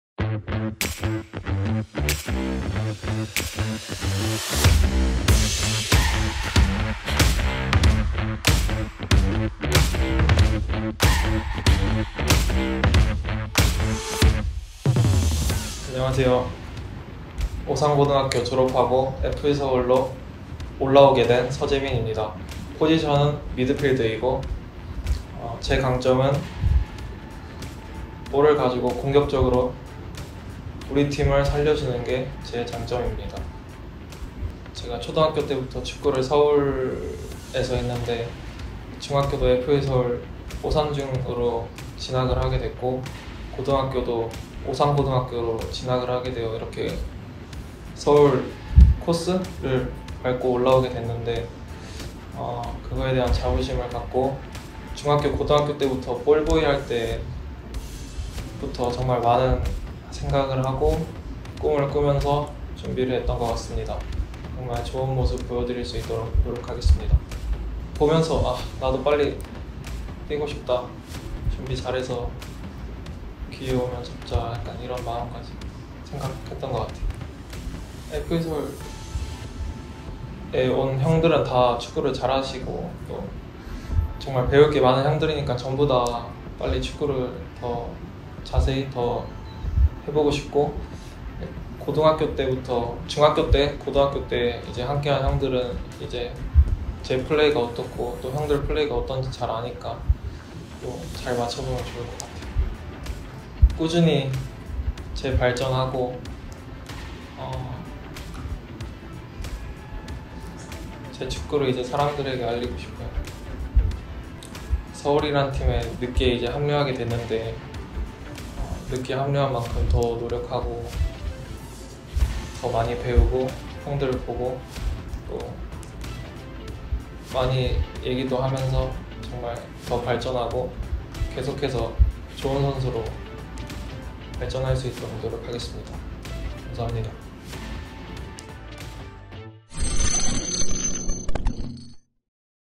MADE IN SEOUL '슈퍼루키' 서재민의 입단 인터뷰
FC서울이 유스 출신 서재민 선수와
프로계약을 체결했습니다.
계약기간은 2024년까지 3년입니다.